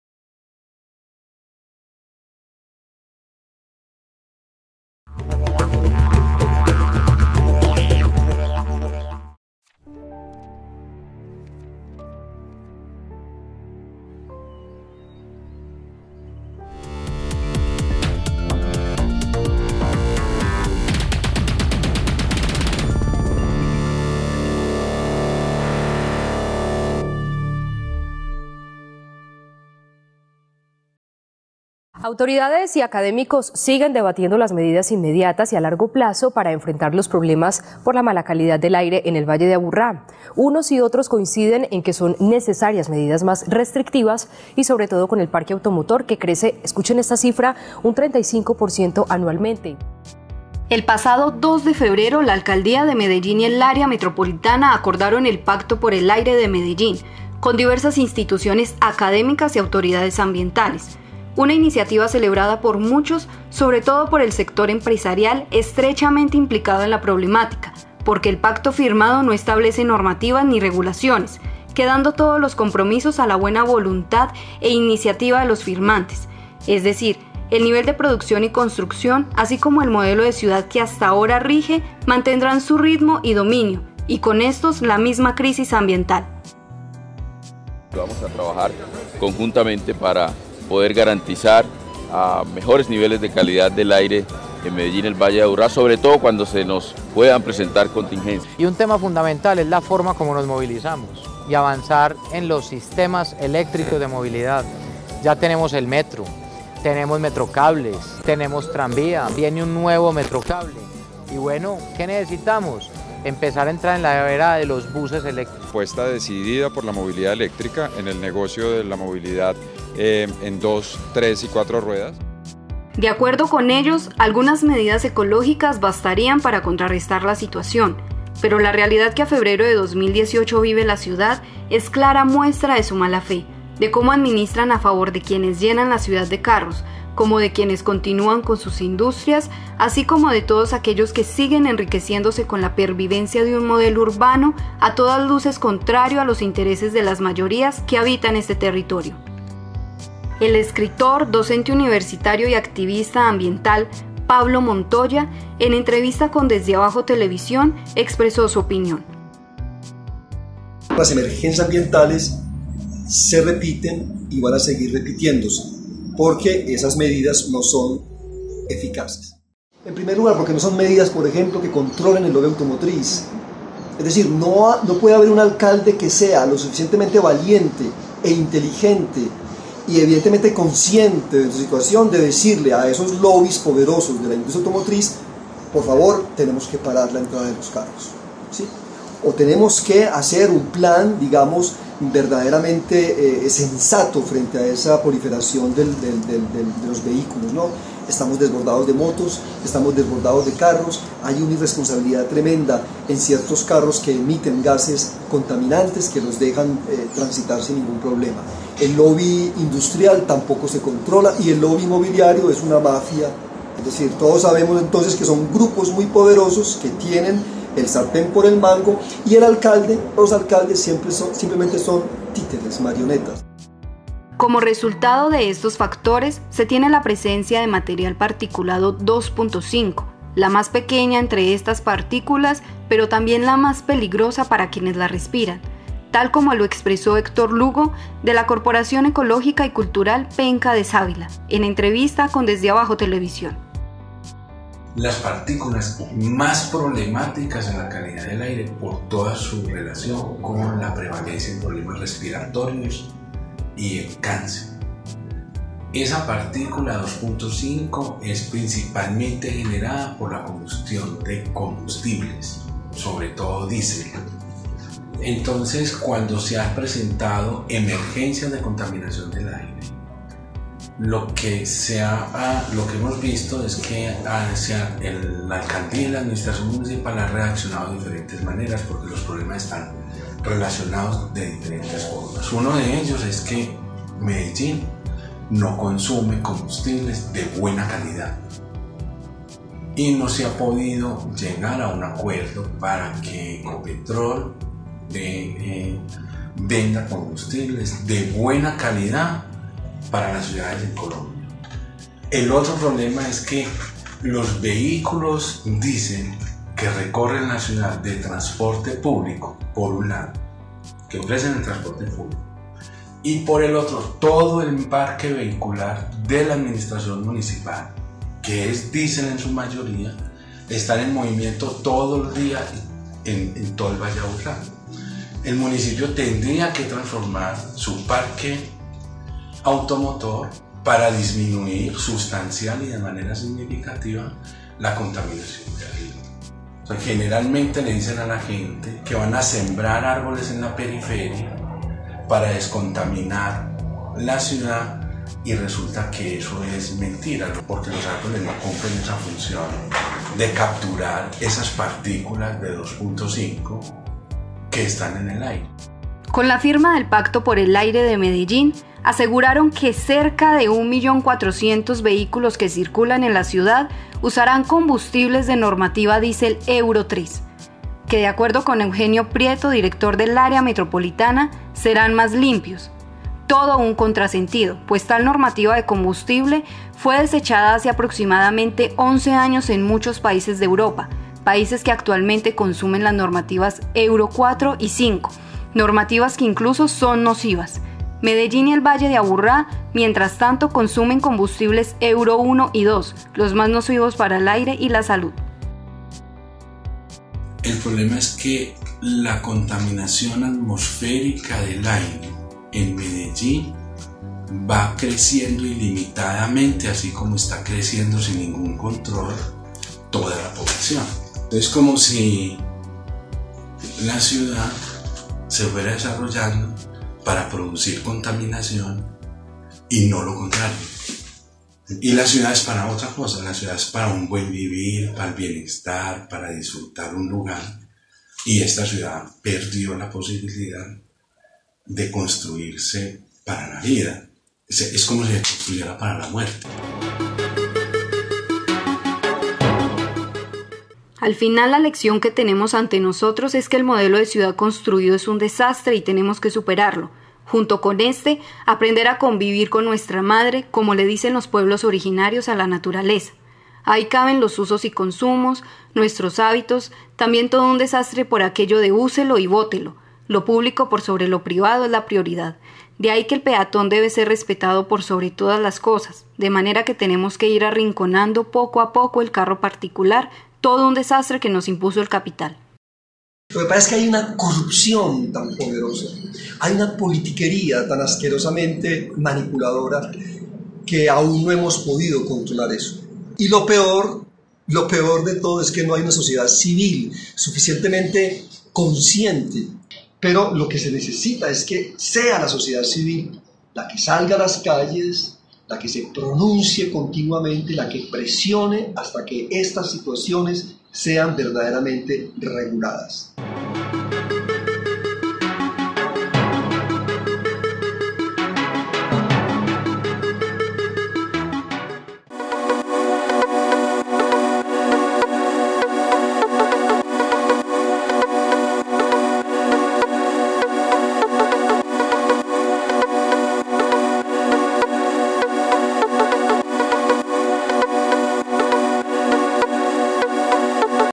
Medellín, crisis ambiental. ¡Ahogados!
Medellín y todo el Valle de Aburrá libran una lucha en contra de la contaminación que amenaza la salud de sus pobladores. Las causas de ésta, corresponden a un modelo de ciudad diseñado para la industria, el comercio y no para la vida; propio de los grandes poderosos del lobby automotriz, industrial e inmobiliario.